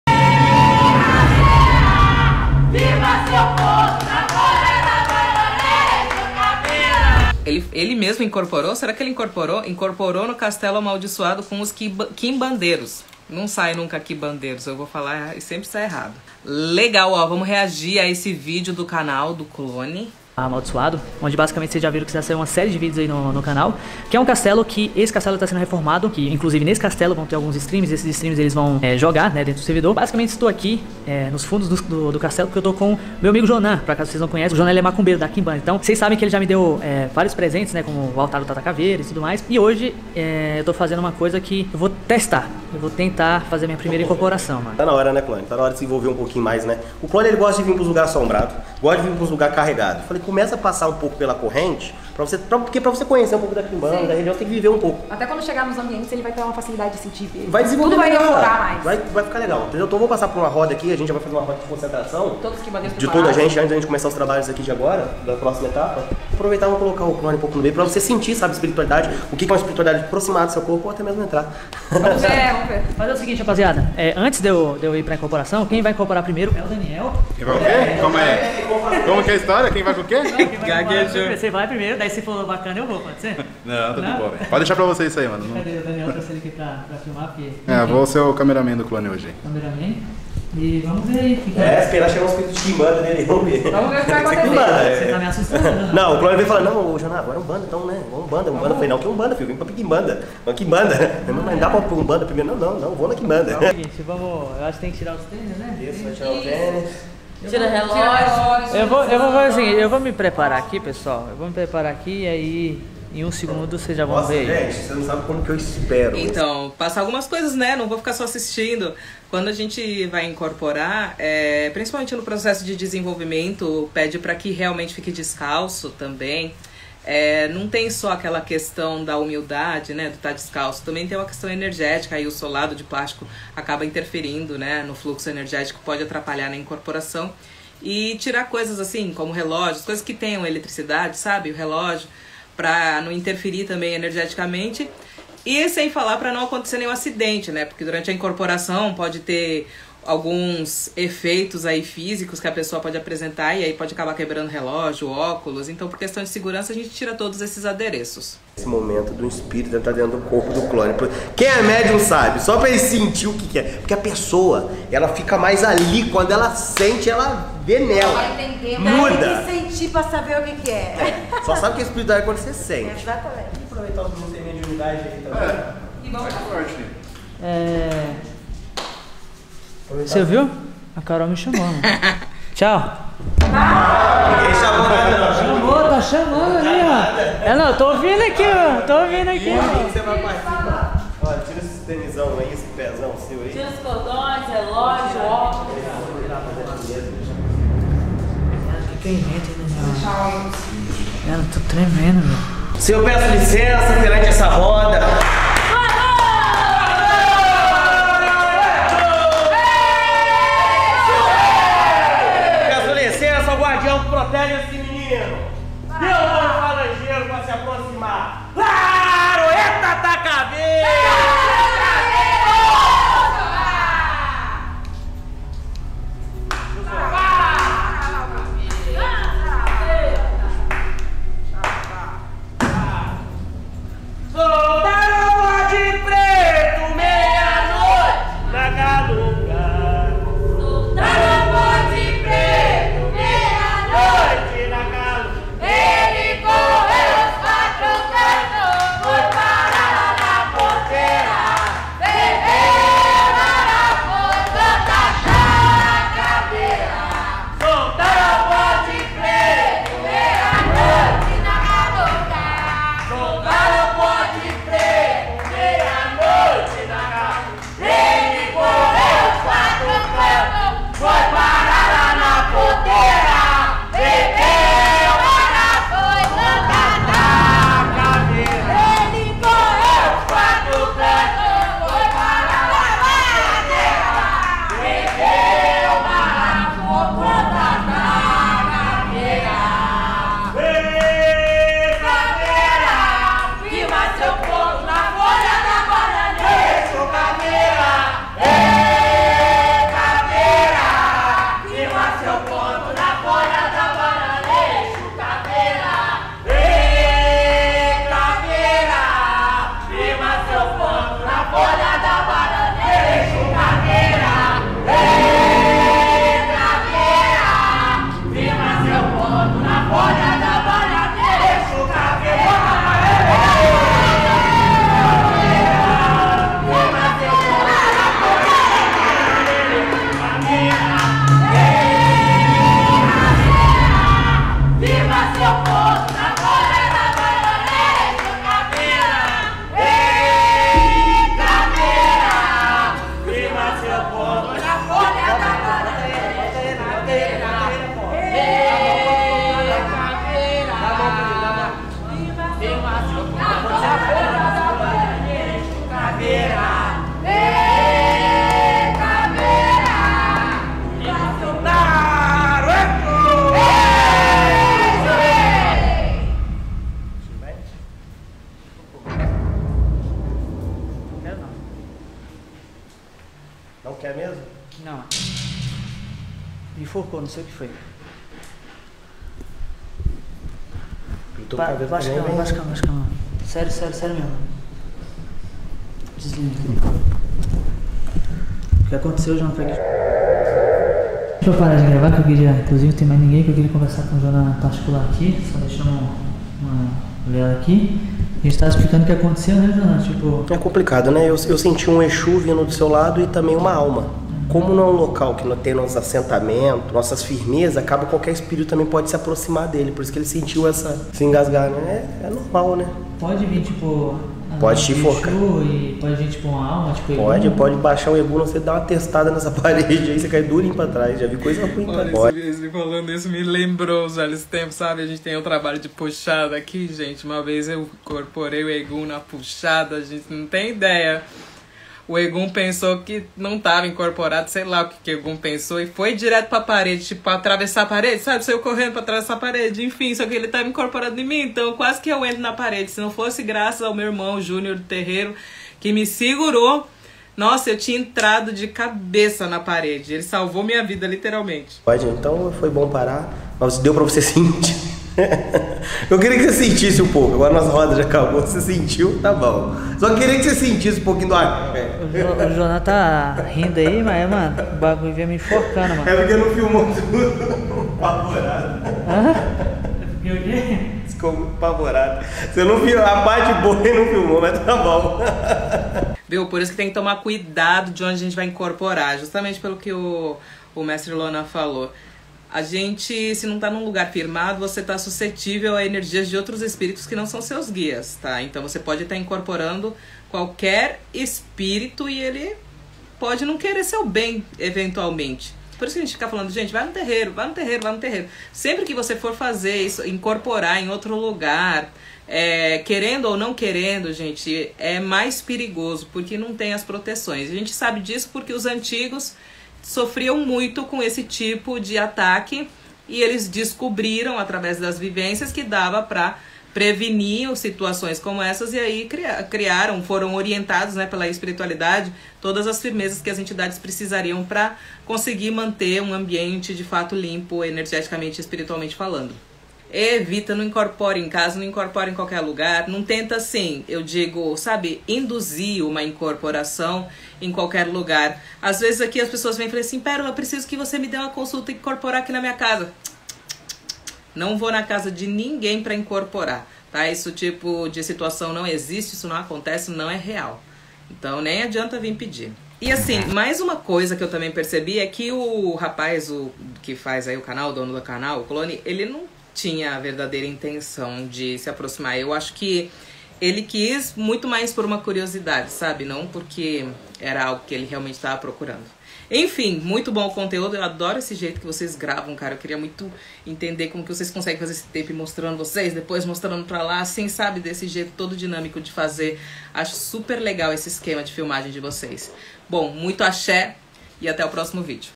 Pérola React Incorporei num castelo amaldiçoado junto com os Quimbandeiros
Ajude na reforma do nosso terreiro em Itanhaém
Pérola